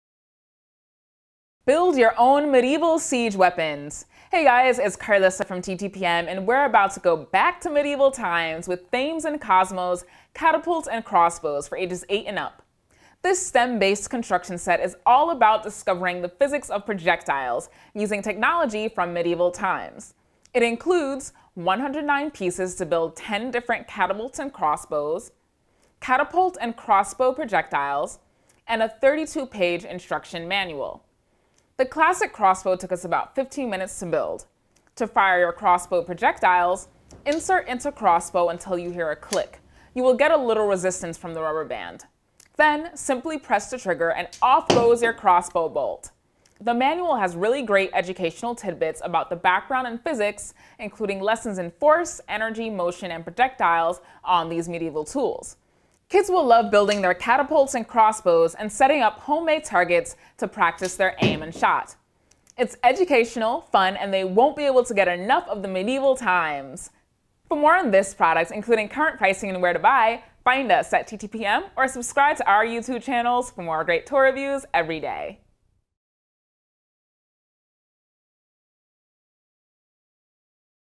Catapults & Crossbows from Thames & Kosmos
Build your own medieval siege weapons with Thames & Kosmos Catapults and Crossbows! This S.T.E.M.-based construction kit allows you to build 10 catapults and crossbows complete with projectiles! For full review and shopping info

Product Info: Travel back to medieval times with Thames & Kosmos Catapults and Crossbows for ages 8 and up. This S.T.E.M.-based construction set is all about discovering the physics of projectiles and using technology from medieval times. It includes 109 pieces to build 10 different catapults and crossbows, catapult and crossbow projectiles, and a 32-page instruction manual. The classic crossbow took us about 15 minutes to build. To fire your crossbow projectiles, insert into crossbow, until you hear a click. You will get a little resistance from the rubber band. Then, simply press the trigger and off goes your crossbow bolt! The manual has really great educational tidbits about the background and physics, including lessons in force, energy, motion, and projectiles, on these medieval tools. Kids will love building their catapults and crossbows and setting up homemade targets to practice their aim and shot. It's educational, fun, and they won't be able to get enough of the medieval times!